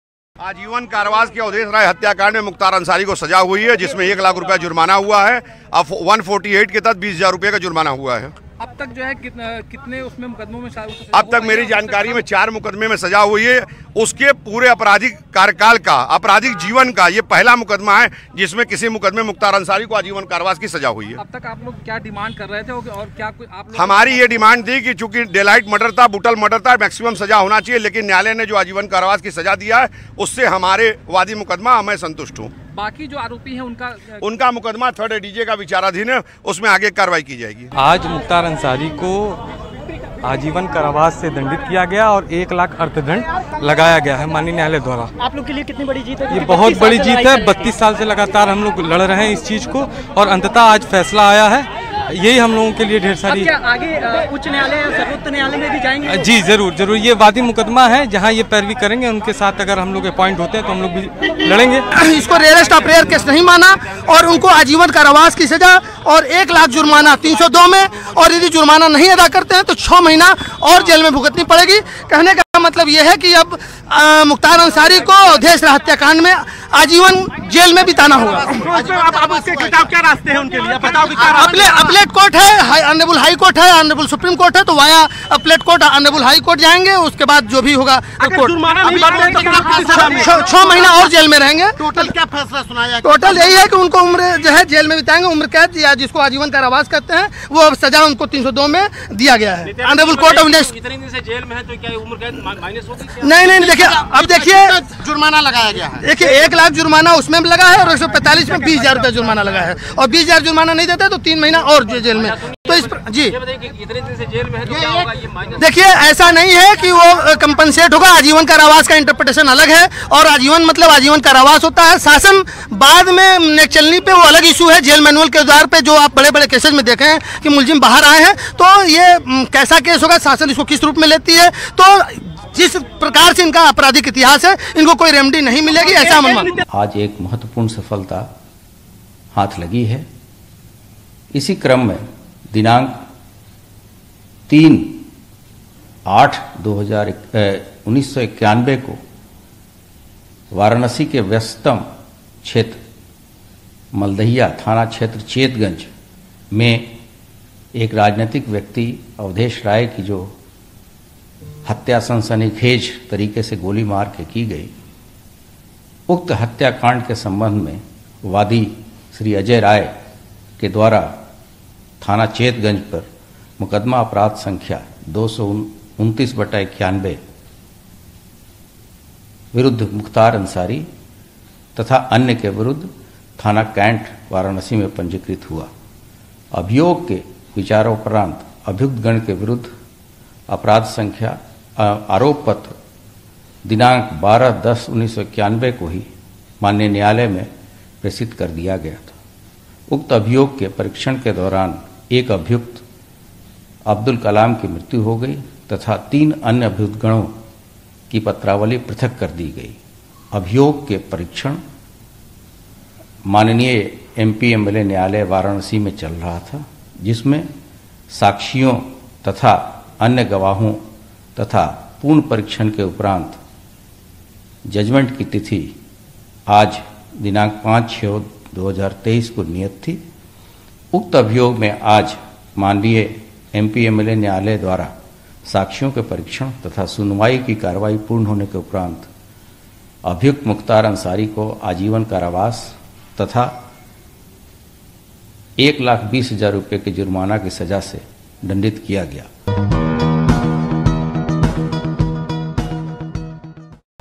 Mukhtar Ansari को कितने Cases में मिली Life Imprisonment सजा, क्या कभी आएगा बाहर...सुनिए वकीलों से
वाराणसी की अदालत से मुख्तार अंसारी को अवधेश राय मर्डर से में उम्रकैद की सजा हुई है। मुख्तार को दो मामलों में सजा सुनाई गई है। इसमें एक केस में 1 लाख रुपये और दूसरे में 20 हजार का जुर्माना लगा है। 3 अगस्त 1991 को वाराणसी के लहुराबीर इलाके में हुई घटना के 32 साल बाद वाराणसी की अदालत ने इस मामले में अपना फैसला दिया है।